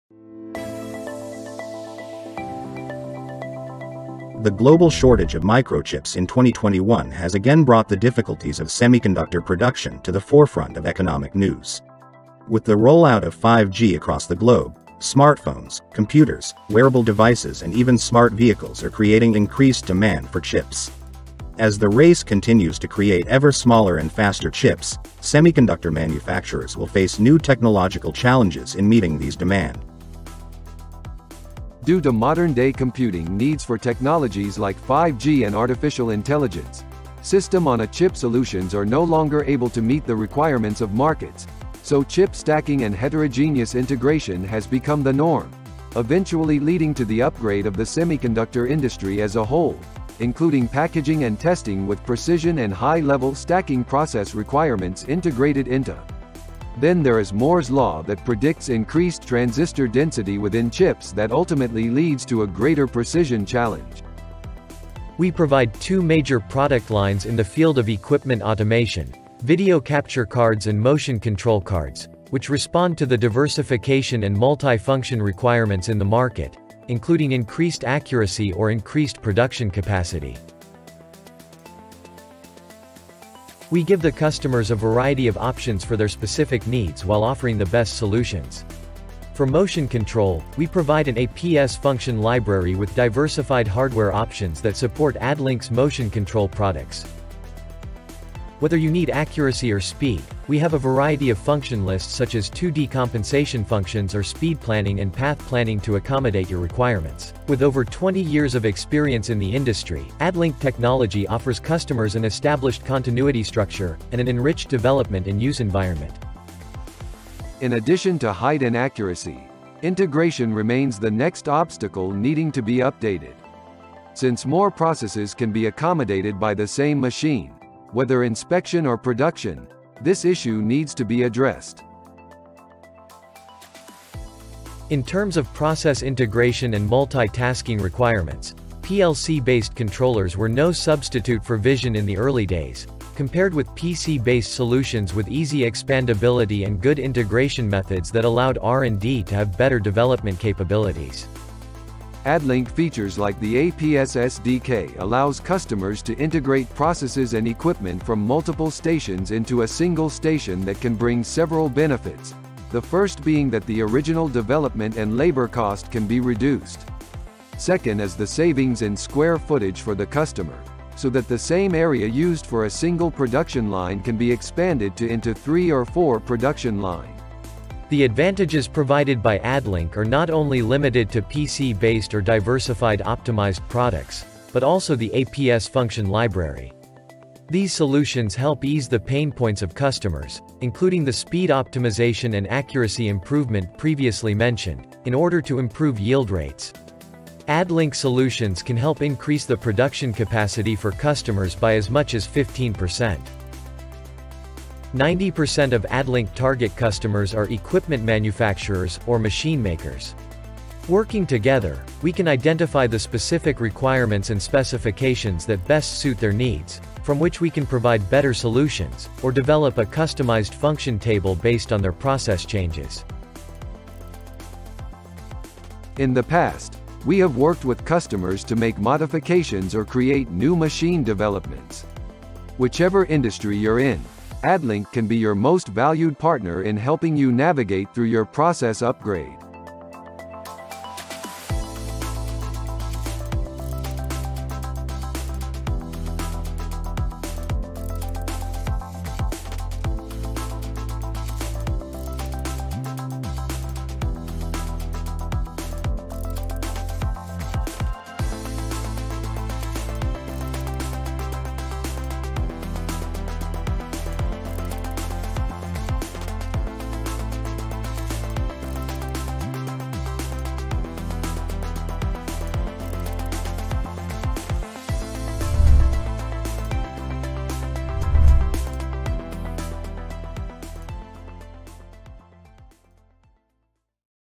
Machine Vision x Motion Control | ADLINLK's Secret Weapon in Semiconductor Packaging and Testing
Semiconductor high-end packaging and testing moves to 3D, ADLINK combines "machine vision" and "motion control" to overcome precision challenges.

Benefiting from the rapid development of 5G, AI, and IoT applications, the semiconductor industry has recently shown strong growth momentum. However, Why is it more difficult for semiconductor high-end packaging and testing to accurately locate? In addition to solving the issue of the smaller and uneven appearance of the object under test in the 5G era, it is also necessary to overcome a small number of diverse trends, the need for frequent machine changes and program changes. Therefore, ADLINK introduces an expandable PC-based motion control technology combined with machine vision functions into the high-end packaging and testing field to meet the industry's high requirements for chip diversity, accuracy and speed.